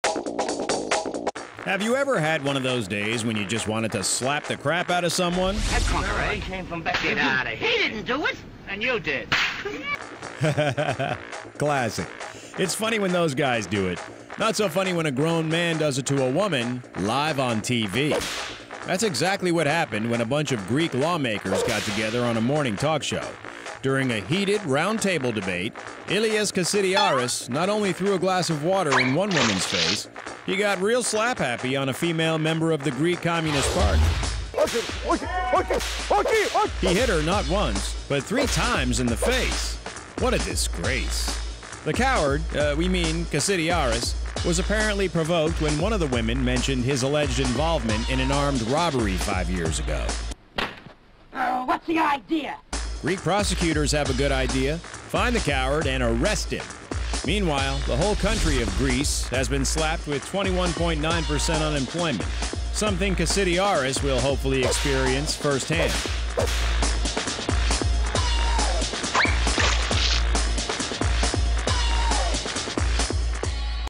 Greek politician slaps woman on live TV
A Greek politician will likely find himself on the wrong side of the law, after he slapped a female rival on live TV.